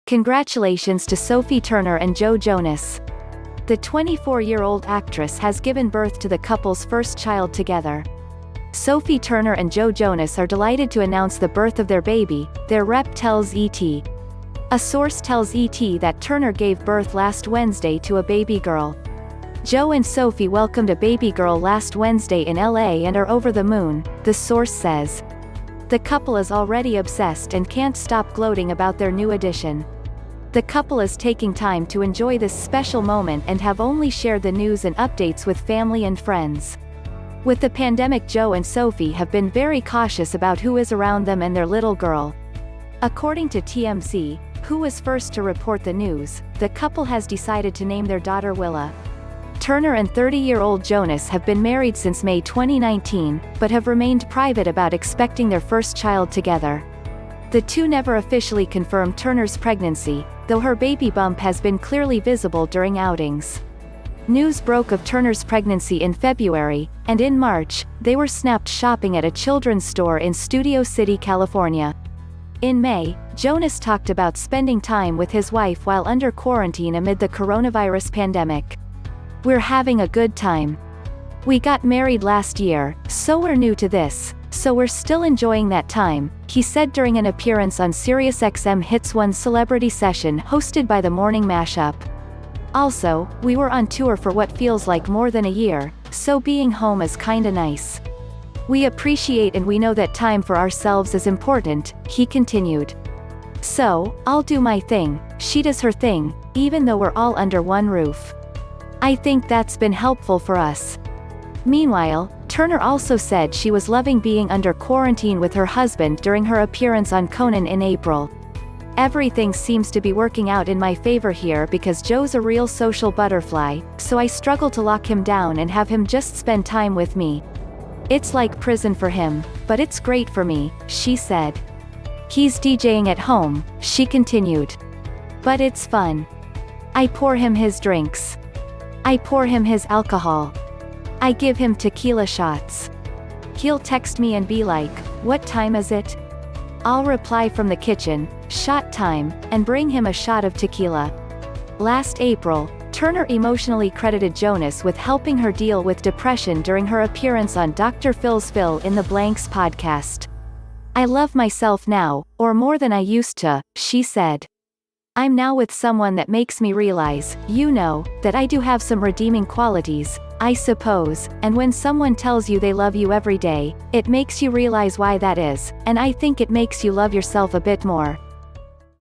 Sophie Turner and Joe Jonas Welcome First Child Together
Congratulations to Sophie Turner and Joe Jonas! The 24-year-old actress has given birth to the couple's first child together.''Sophie Turner and Joe Jonas are delighted to announce the birth of their baby, their rep tells ET. A source tells ET that Turner gave birth last Wednesday to a baby girl.''Joe and Sophie welcomed a baby girl last Wednesday in L. A. and are over the moon,'' the source says. ''The couple is already obsessed and can't stop gloating about their new addition. The couple is taking time to enjoy this special moment and have only shared the news and updates with family and friends. With the pandemic Joe and Sophie have been very cautious about who is around them and their little girl.''According to TMZ, who was first to report the news, the couple has decided to name their daughter Willa. Turner and 30-year-old Jonas have been married since May 2019, but have remained private about expecting their first child together. The two never officially confirmed Turner's pregnancy, though her baby bump has been clearly visible during outings. News broke of Turner's pregnancy in February, and in March, they were snapped shopping at a children's store in Studio City, California. In May, Jonas talked about spending time with his wife while under quarantine amid the coronavirus pandemic.''We're having a good time. We got married last year, so we're new to this, so we're still enjoying that time,'' he said during an appearance on SiriusXM Hits 1's Celebrity Session hosted by The Morning Mash Up. ''Also, we were on tour for what feels like more than a year, so being home is kinda nice.''''We appreciate and we know that time for ourselves is important,'' he continued. ''So, I'll do my thing, she does her thing, even though we're all under one roof. I think that's been helpful for us.''Meanwhile, Turner also said she was loving being under quarantine with her husband during her appearance on Conan in April.''Everything seems to be working out in my favor here because Joe's a real social butterfly, so I struggle to lock him down and have him just spend time with me... It's like prison for him, but it's great for me,'' she said.''He's DJing at home,'' she continued. ''But it's fun! I pour him his drinks. I pour him his alcohol. I give him tequila shots. He'll text me and be like, 'What time is it?' I'll reply from the kitchen, 'Shot time,' and bring him a shot of tequila.''Last April, Turner emotionally credited Jonas with helping her deal with depression during her appearance on Dr. Phil's Phil in the Blanks podcast.''I love myself now, or more than I used to,'' she said. ''I'm now with someone that makes me realize, you know, that I do have some redeeming qualities, I suppose, and when someone tells you they love you every day, it makes you realize why that is, and I think it makes you love yourself a bit more.